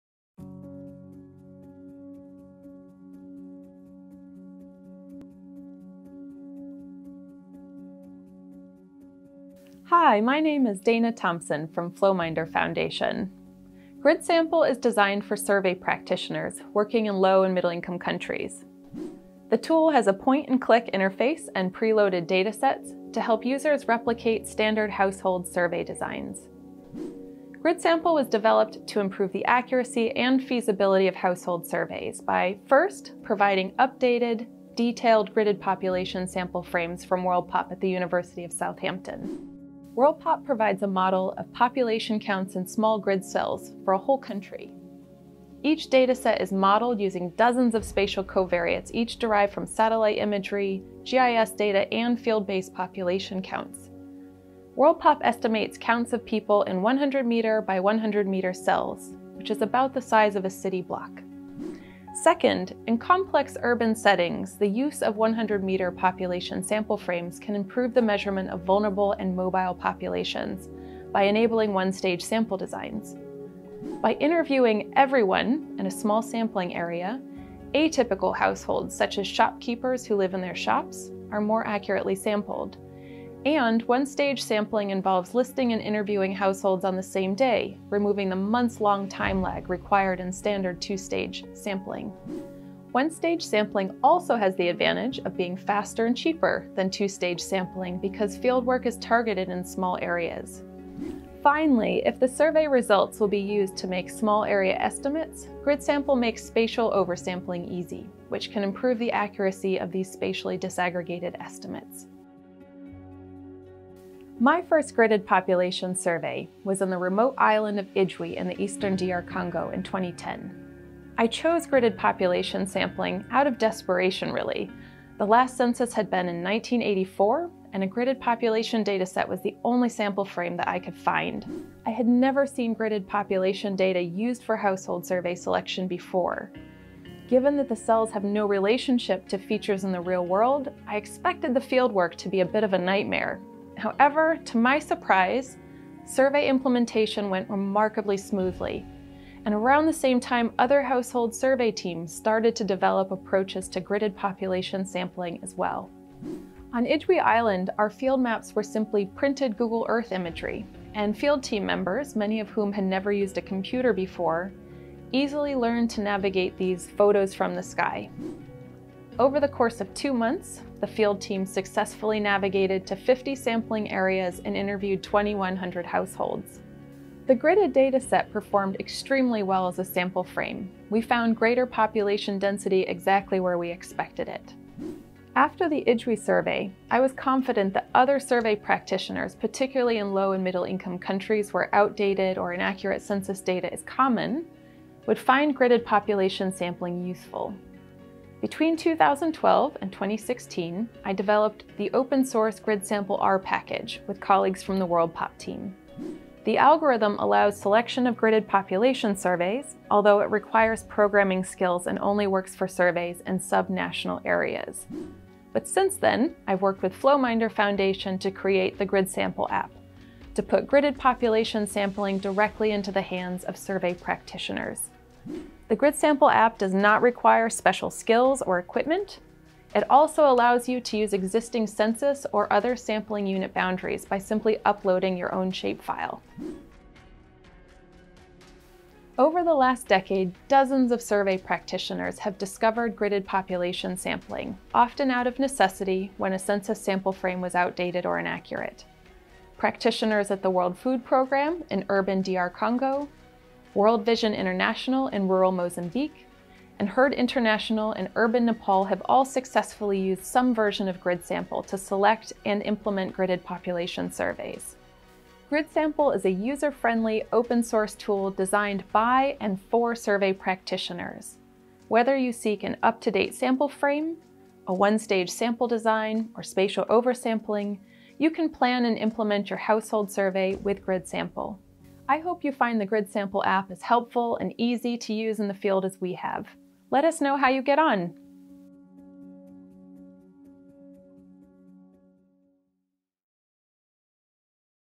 GridSample: A tool to select gridded population survey samples
Find out more about GridSample, Flowminder Foundation's user-friendly tool to select household survey sampling areas in complex settings.